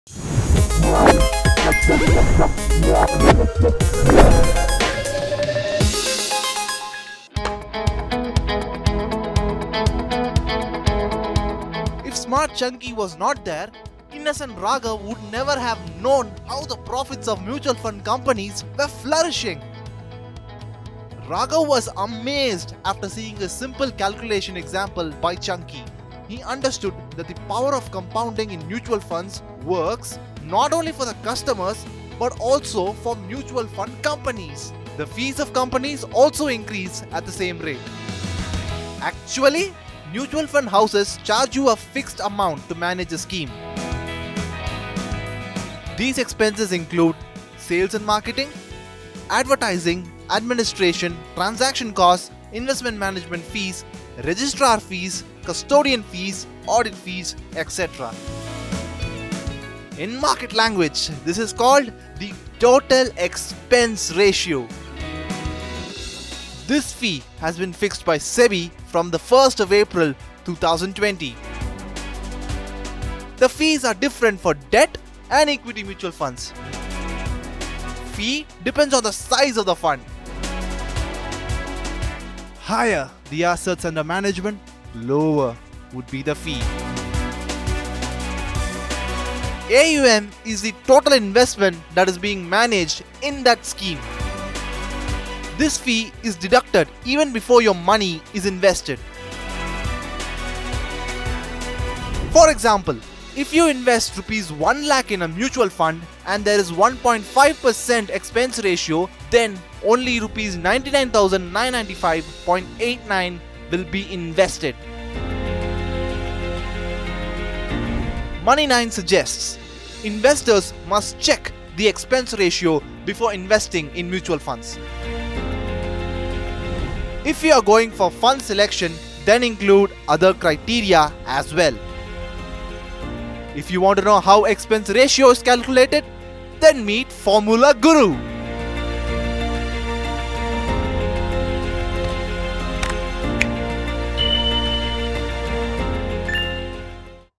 EP 13: 2+2=5 | How mutual fund companies make such huge profits? | Money9 English
The power of compounding in mutual fund investing is not just for the investors. The fees of companies also increase at the same pace.